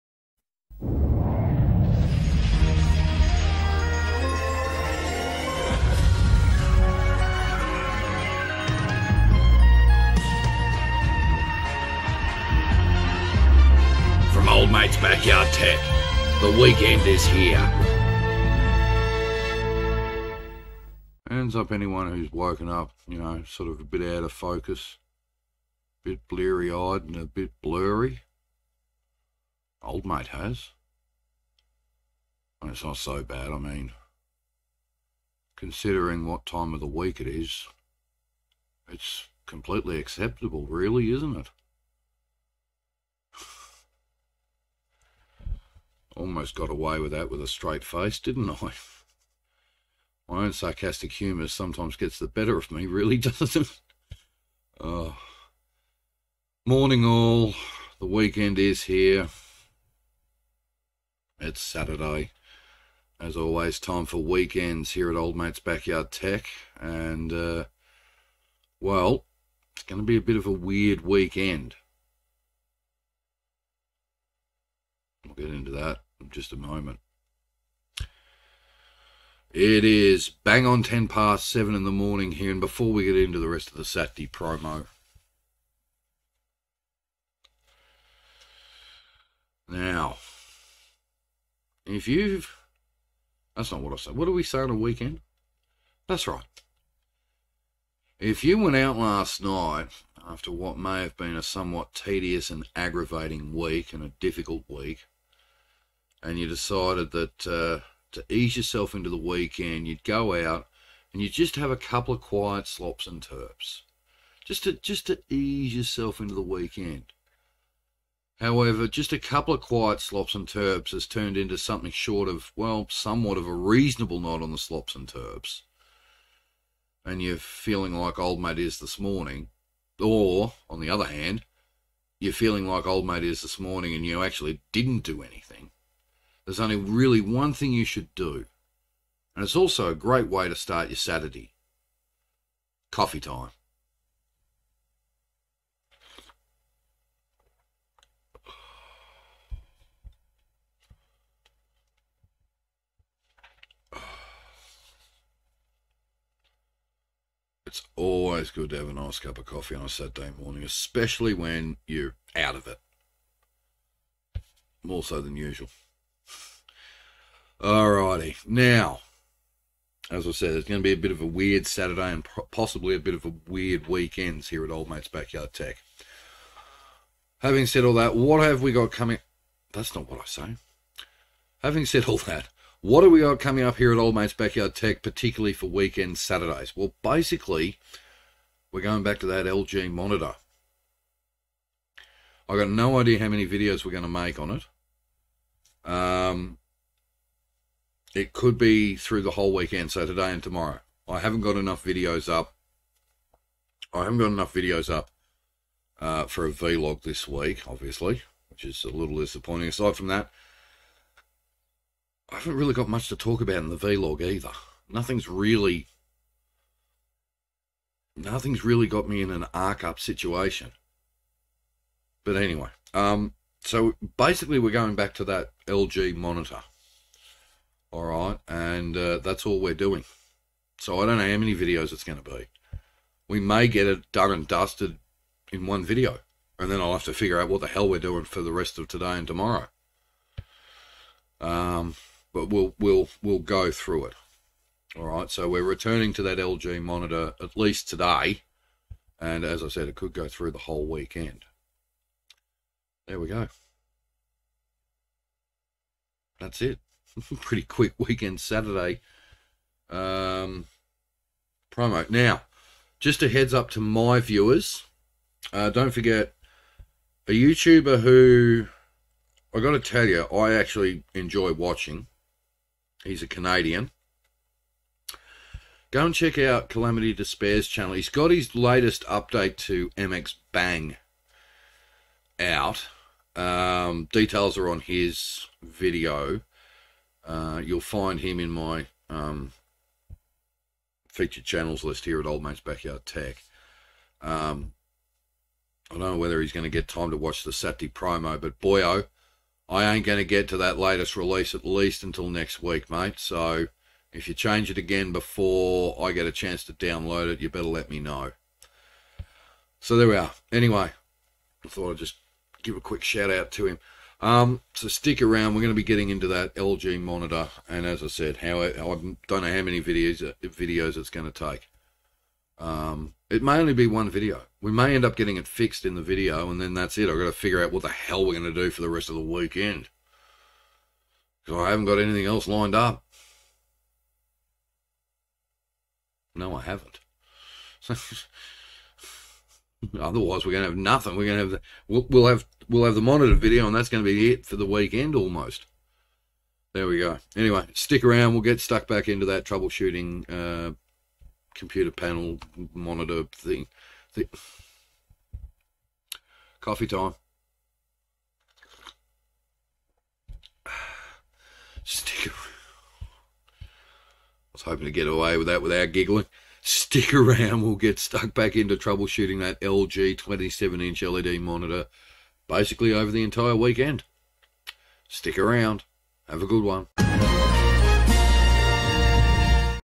Weekend Promo - Saturday - Not Sure How Many Videos
The weekend is here it's Saturday and not sure how many videos are on the cards, watch today's promo to see why

Other Music used in this video
Ident used in this video is a mock made by me of the GTV 9 Saturday night at the movies from 1993